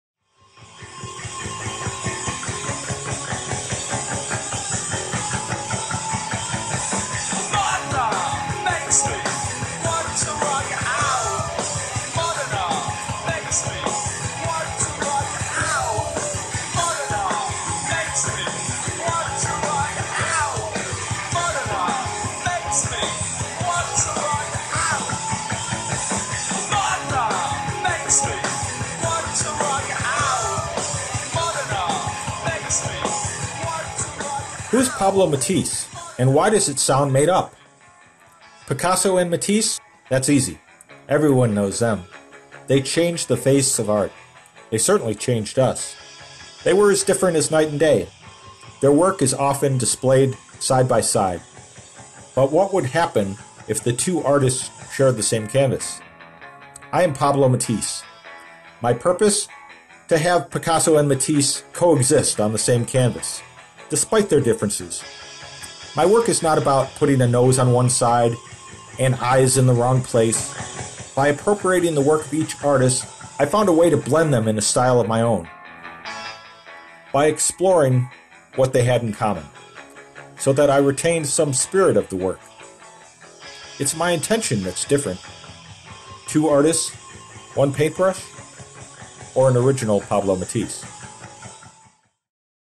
What if Matisse and Picasso were secretly the same painter?
Pablo Picasso famously said, “Good artists copy. Great artists steal.” Not wishing to disappoint, I have taken the name Pablo Matisse, riffing on Pablo Picasso and Henri Matisse in a series of appropriated works that are unique and not reducible to either of them, or both together.
Pablomatisse.com